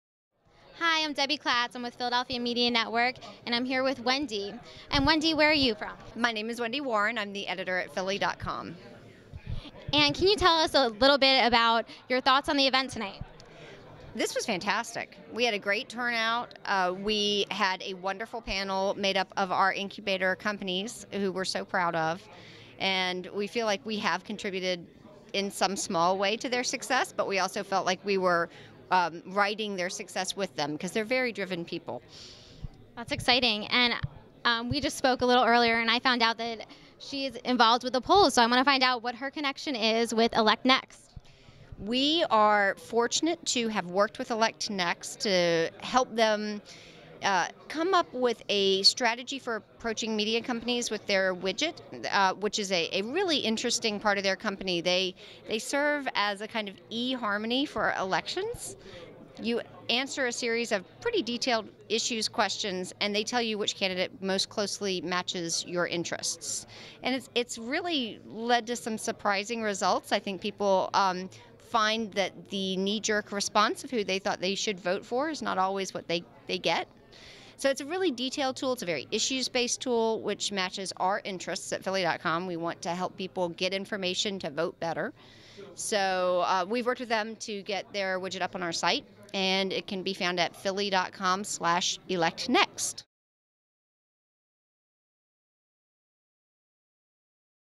Philly.com Editor on the Incubator Project
Project Liberty Enters second year.  Move to combined space should be interesting to keep an eye on.  Video by Jay Treble sound Roy Jak  and Barbara Adams.